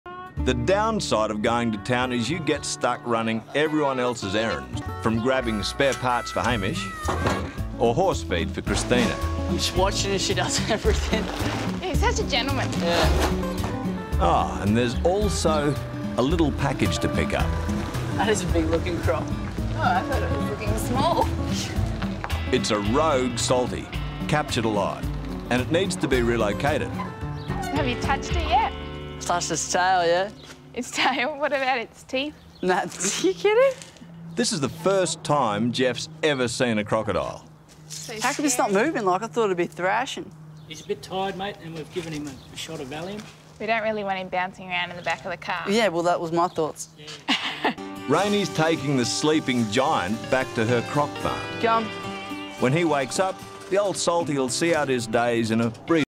Just A Normal Day In The Outback, Picking Up A Half-Tonne Croc! 🐊
Jeff and Rainie need to pick up a rogue saltwater crocodile, just a normal day in outback northern territory!